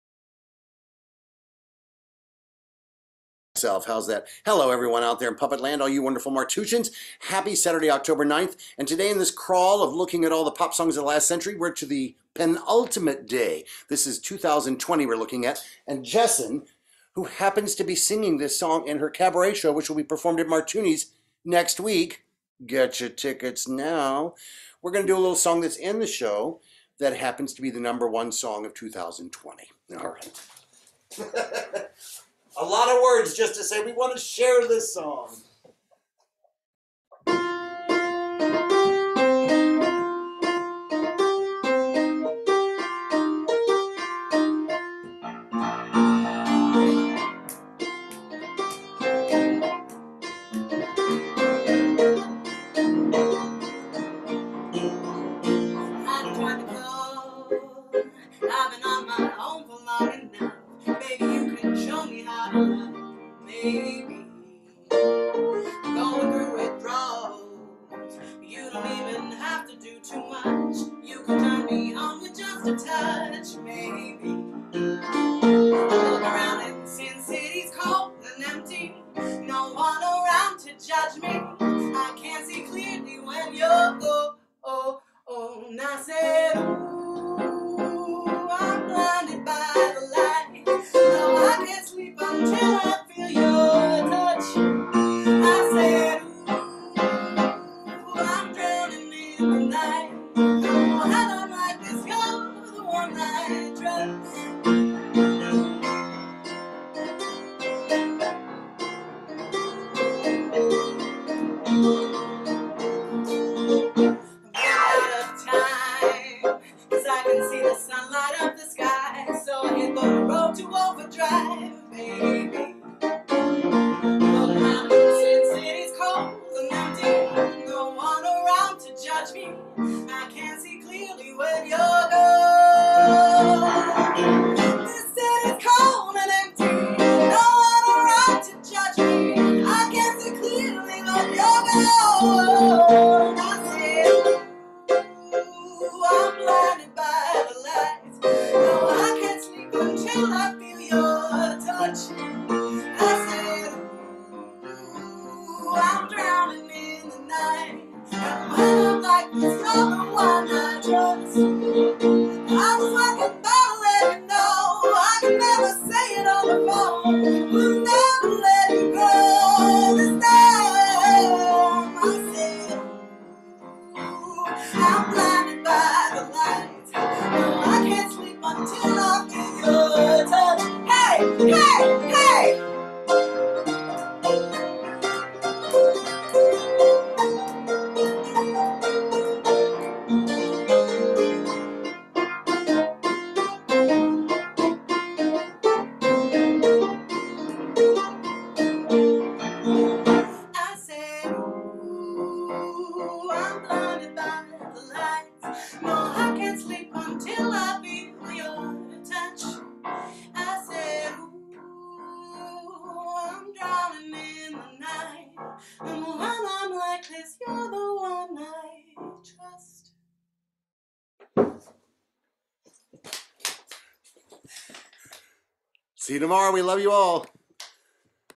2020: The #1 song of the year
2020:  The #1 song of the year, graciously sung by Jessen Langley.